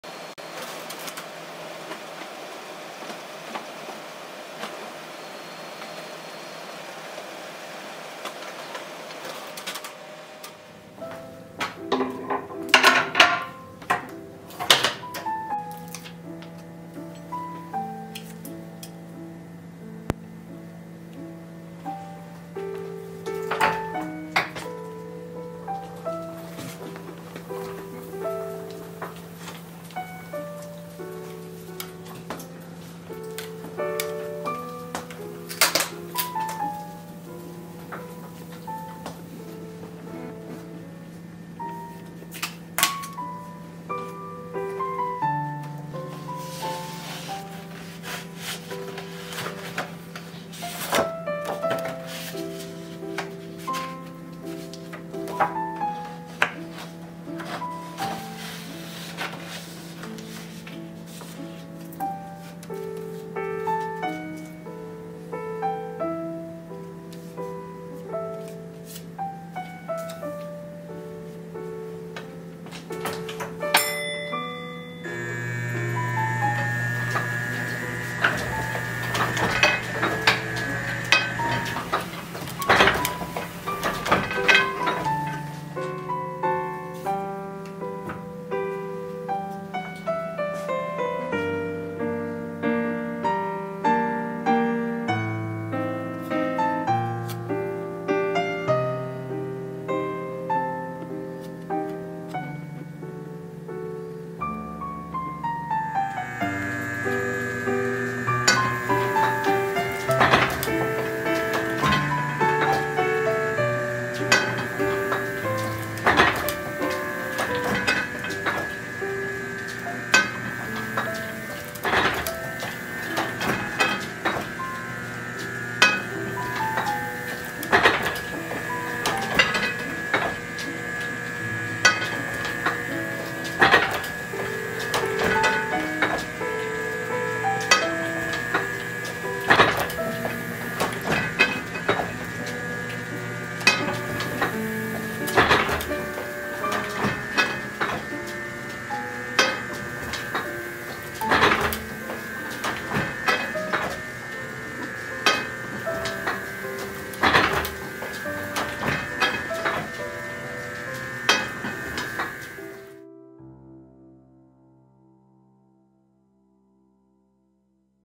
Semi_Letterpress Business card.쿠션지 양면인쇄 후 레터프레스인쇄 명함만들기.
쿠션지335g용지에 양면인쇄 후 단면 레터프레스 고급명함을 제작했습니다.
I Love Letterpress _ 왈츠디자인